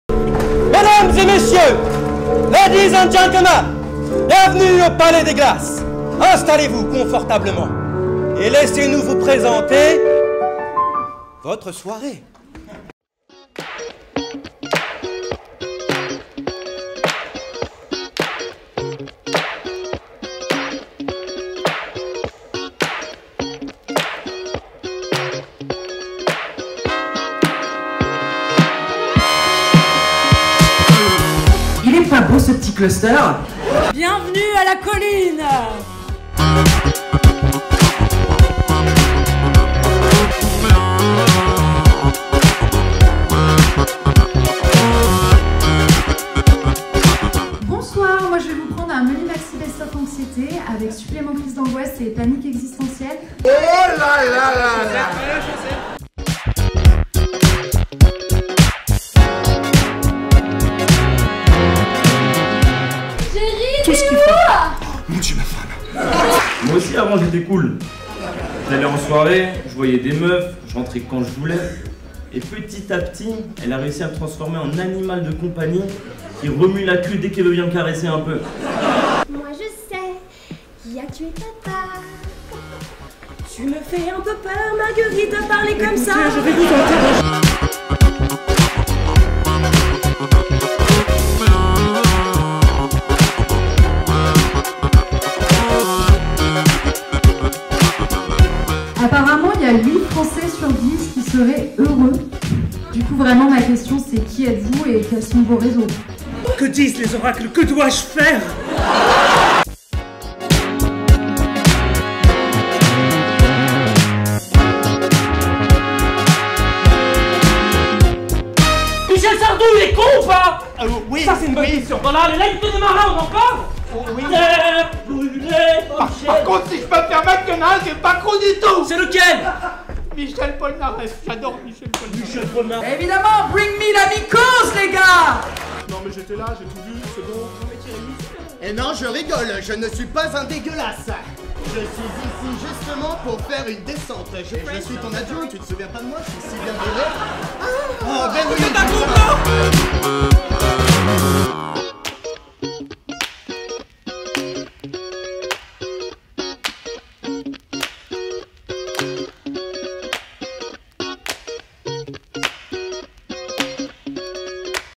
CLIP IMPRO PARTY 2022 | Cours Paul Clément
Pour ceux qui n'ont pas le temps de regarder les 2h de Soirée d'Improvisation, voici un résumé des meilleurs moments ! 🥳